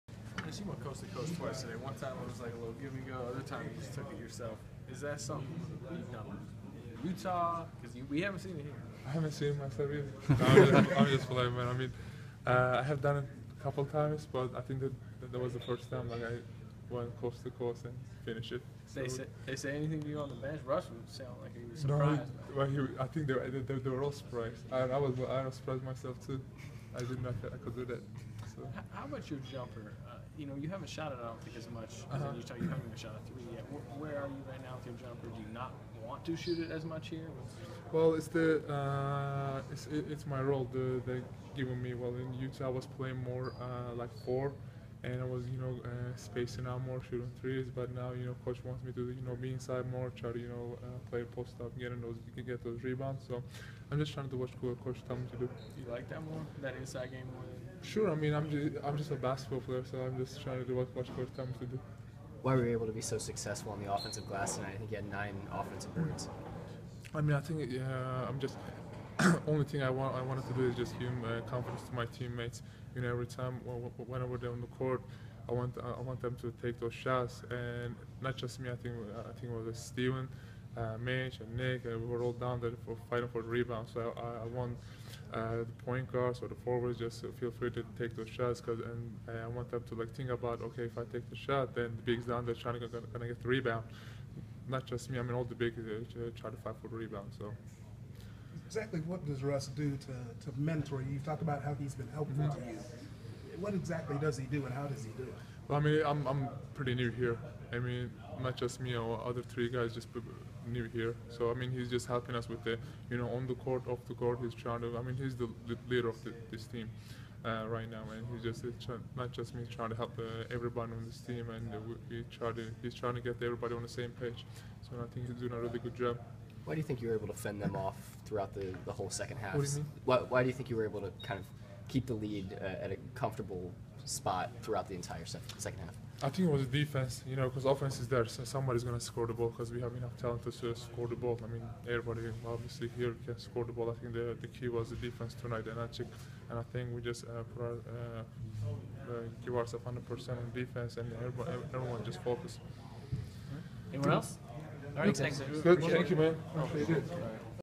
Enes Kanter on his big night and Russell Westbrook
Enes talks about his coast to coast play and also talks about Russell Westbrook's triple double.